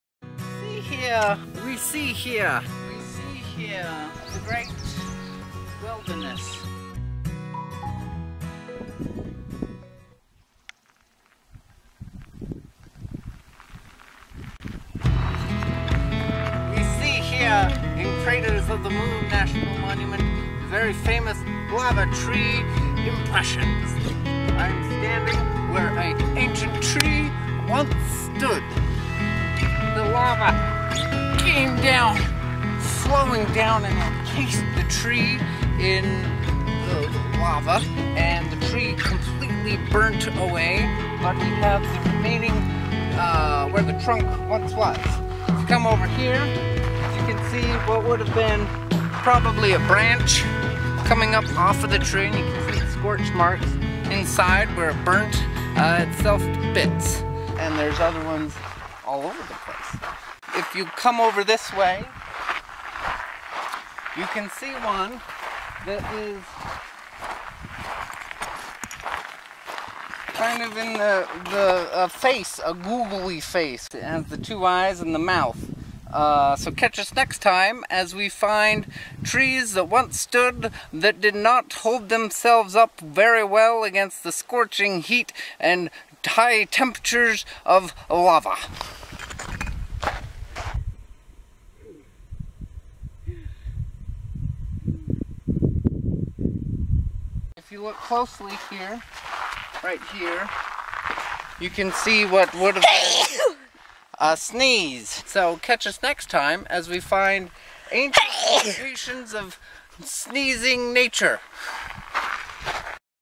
Lava Trees in Craters of the Moon National Monument
WE SEE HERE THE TREE MOLDS on the Wilderness Trail in CRATERS OF THE MOON NATIONAL MONUMENT. These are ancient trees that were encased in lava and left behind their mold in the time capsule of basalt.

CRATERS OF THE MOON NATIONAL MONUMENT.

Tucked in the eastern centralish location of Idaho is the national monument of Craters of the Moon. This is not a large park, but it has many unique features that are all withing very close proximity of each other. Lava cones, lava domes, spatter cones, ah-ah flows, pahoehoe flows, lava tubes, lava caves, lava lakes, the blue dragon flow, and the very rare tree molds.

If you are traveling south from Yellowstone or the Grand Tetons divert your path through Idaho Falls and head over to Craters of the Moon and take in this small national monument for an experience that is other worldly.

THE TREE MOLDS.

The tree molds were formed when an ancient lava flow came down from the mountain behind us in the video and flowed into an ancient grove of trees. The lava encased the trunks of the trees. The formidable trees did not completely give way to the heat and force of the lava, but ultimately died due to the intense heat and scorching of their outer layers. Over time the trees decayed and eroded away leaving behind the mold of their former trunks and low branches in the remaining basalt.

The lava tree formations in this video are all of the trunks of threes as they were when they were standing. On the Tree Molds Trail about half mile out from the parking lot there are molds of trees that toppled over and left their impression on the top of the lava flow. These ones are pretty cool because they left the impression of their bark in the basalt! Check them out!

THE TREE TREES HIKE.

The tree molds (also known as lava trees) that made it into this video are out about 2 miles on the wilderness trail at the southern most extent that is drive-able in the park. There is not much shade and the volcanic pumice that scatters the ground (you can hear the crunch of it in the video when we walk on it) absorbs the heat of the sun very well, radiating that heat back up. On the day we hiked out to the lava trees the air temperature was only about 89 degrees, however with the black ground and the heat that was coming off of it, the temperature felt more like 110. Make sure to have plenty of water, and start your hike early in the morning or later in the evening to avoid the heat.

Check out other hikes at Craters of the Moon National Monument

WE SEE HERE TRIVIA!
The grass and small flowers were really getting to Melissa, her allergies kicked in pretty hard on this wilderness trail to the tree molds.